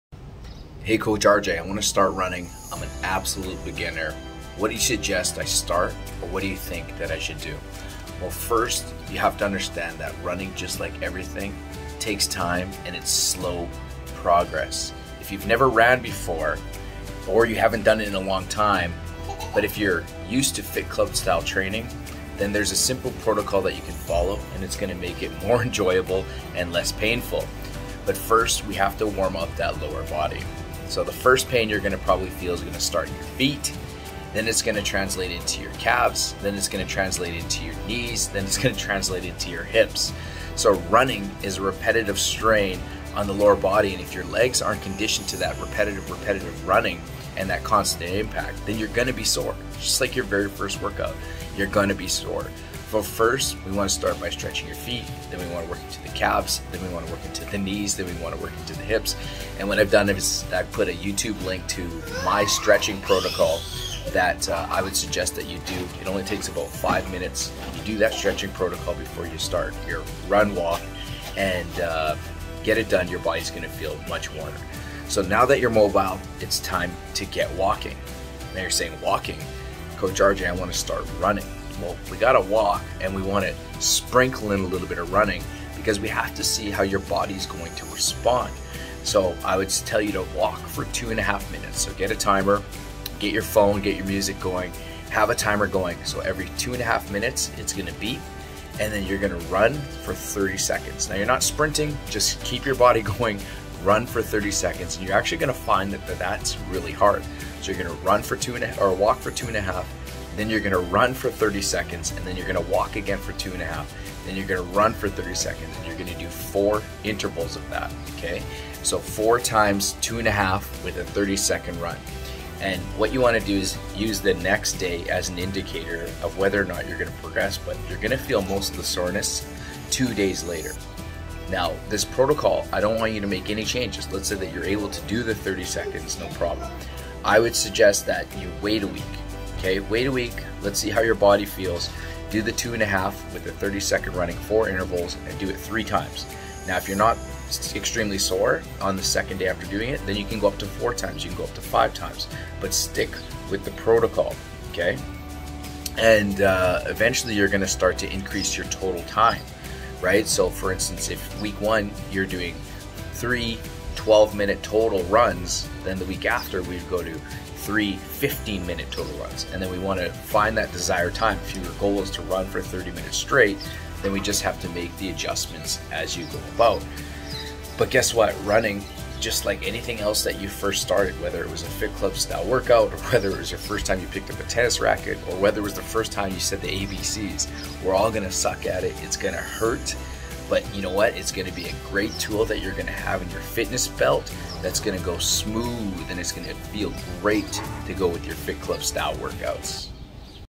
I want to start running, I am an absolute beginner, where should I start?
Running just like everything requires time and slow progress.

If you have never run before, or haven’t run in a long time, but you are used to Fit Club style training…

Then here’s a simple protocol.

First, warm up your lower body.

The first pain you will feel is probably your feet, then your calves, your knees then your hips.

Running is a repetitive strain on the lower body and if your legs aren’t used to the constant impact, they will be sore.

So start by stretching your feet, and calves, mobilizing the knees and loosening your hips.

I put a link for a quick mobility routine you can do before you start in the description

Now we’ve got your mobile, it's time to get you walking.

Walking!?  Yes, we have to walk before we run.

Walk for 2.5 mins, then run for 30 seconds.

Repeat this 4 times and that’ll get you 12 mins of movement.

Use the next day as an indicator if you can progress or not, but I would suggest doing this 3 times for 1 week then progress your running times weekly.

Eventually stretching your total time of running longer by 3 mins.

After your run/walk you should repeat the stretch routine I gave you.

Running takes time and you will suck at it.

But it is a great tool to have under your fitness belt along with Fit Club Style training.